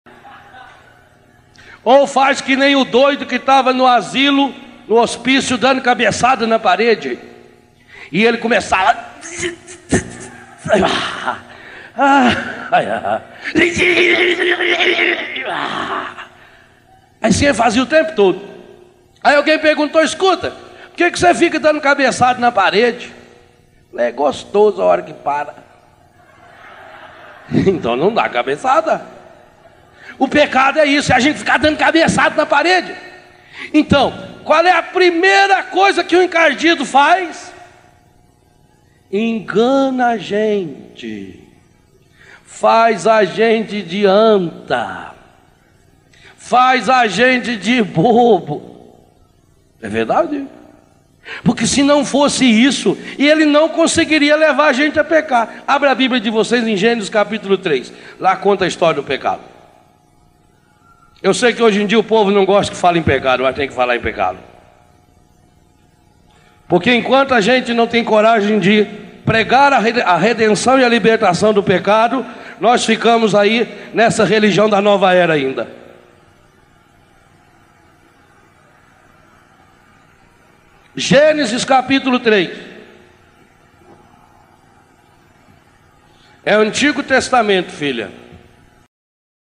Faz que nem o doido que estava no asilo, no hospício dando cabeçada na parede, e ele começava, tum,
Faz que nem o doido que estava no asilo, no hospício dando cabeçada na parede, e ele começava, tum, tum, tum, ahhhhh, ahahahahah, e. tum, tum, ahhhhhh, e isso ele fazia o tempo todo; aí alguém perguntou, escuta, por que você fica dando cabeçada na parede? falou, é gostoso a hora que para, ahahah, então não dá cabeçada; o pecado é isso, é a gente ficar dando cabeçada na parede; então, qual é a primeira coisa que o encardido faz? engana a gente, faz a gente de anta, faz a gente de bobo, é verdade, porque se não fosse isso ele não conseguiria levar a gente a pecar; abra a Bíblia de vocês em Gênesis capítulo 3; lá conta a história do pecado, eu sei que hoje em dia o povo não gosta que fala em pecado, mas tem que falar em pecado, porque enquanto a gente não tem coragem de pregar a redenção e a libertação do pecado nós ficamos aí nessa religião da nova era ainda; Gênesis capítulo 3, é Antigo Testamento, filha,